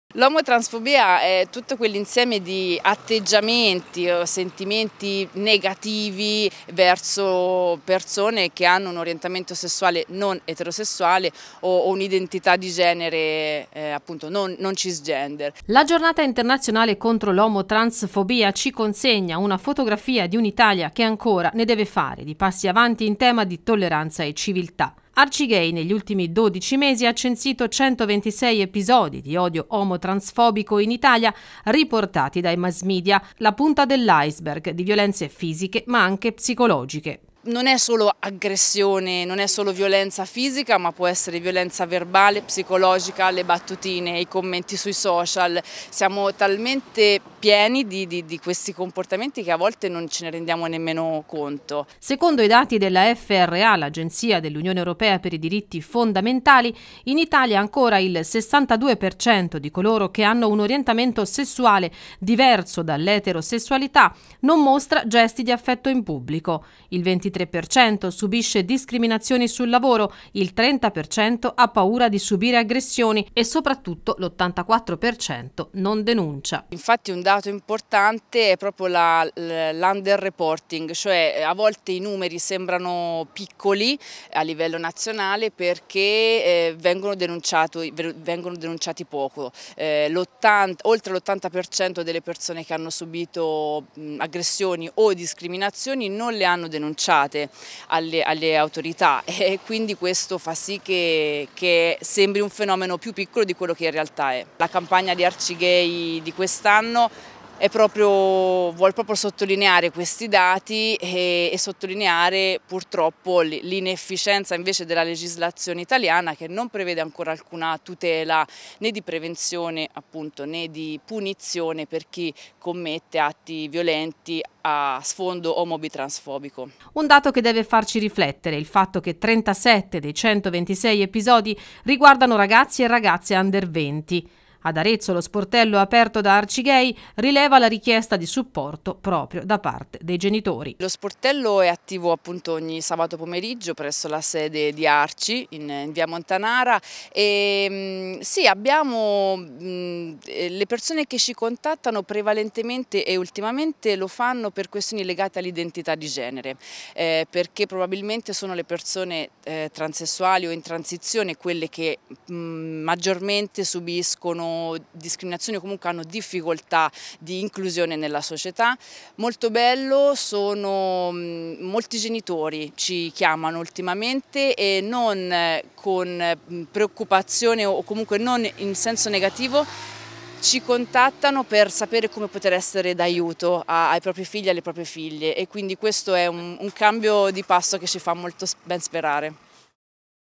Giornata contro l'omotransfobia. Arcigay: "Al nostro sportello tanti genitori."
La giornata internazionale contro l'omotransfobia ci consegna una fotografia di un Italia che ancora ne fare di passi avanti in tema di tolleranza e civiltà.
Secondo i dati della F.R.A., l'agenzia dell'Unione europea per i diritti fondamentali, in Italia ancora il 62% di coloro che hanno un orientamento sessuale diverso dall'eterosessualità, non mostra gesti di affetto in pubblico. Il 23% subisce discriminazioni nel luogo di lavoro, il 30% ha paura di subire aggressioni e soprattutto l'86% non denuncia.
Arcigay, negli ultimi dodici mesi, ha censito 126 episodi di odio omotransfobico riportati dai mass media. La punta dell'iceberg di violenze fisiche, ma anche psicologiche.
Un dato che deve farci riflettere è il fatto che 37 dei 126 episodi censiti riguardino ragazzi e ragazze under 20. Ad Arezzo lo sportello aperto da Arcigay rileva la richiesta di supporto proprio da parte di tanti genitori.